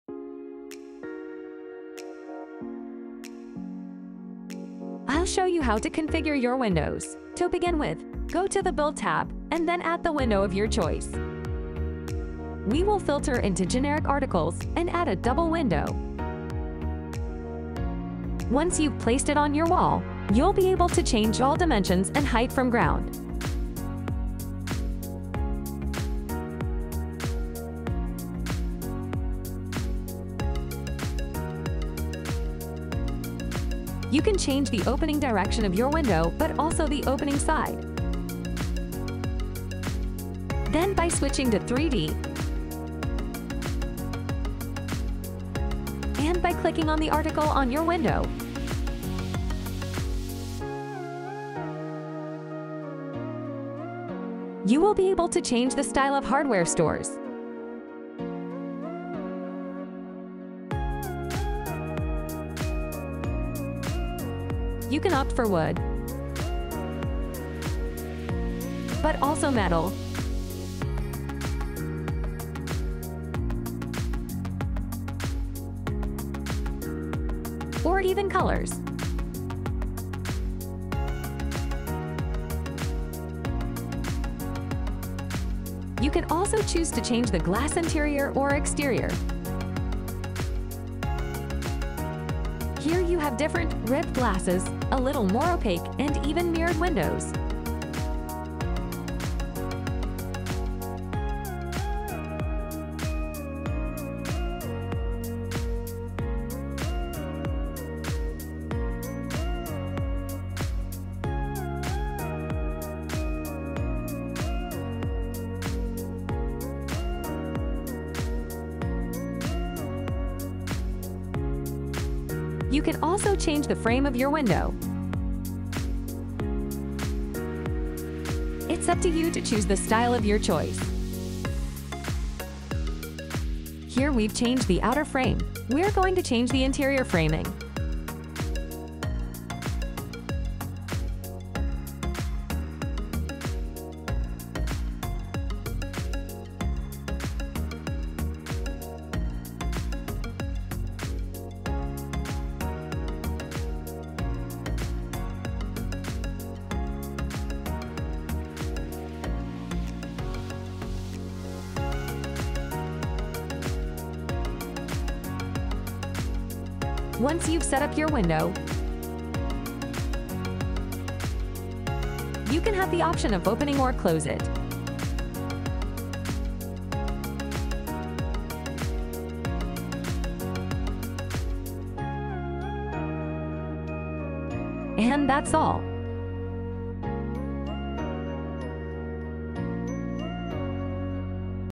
How to use the window settings? | HomeByMe Tutorials
In this tutorial with HomeByMe we will sho you how to use the window settings in order to customize your windows starting from the the measurements to the opening side of your windows, all you have to do is to add the window of your choice, place it on the wall and then click on it and on edit to be able to adjust and customize all the details in the window.

Do you want to make your own interior design project, easily and for free?